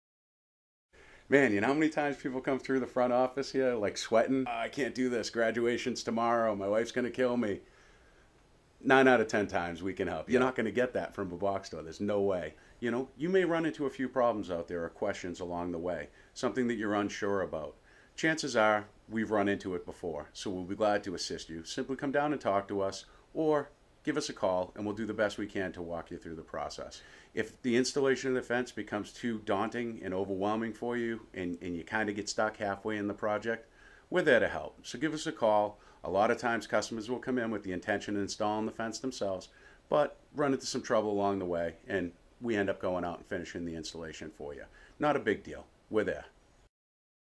We Help if you get Stuck
As long as it's an Arrow Fence product we will help you if you get stuck. If you're a contractor or a Do It Yourself(er) we are will get you out of any jam. We've been installing fence since 1969 so we've encountered almost every problem there is. Best part is we've found a way easier solution than you are thinking. We're there for you.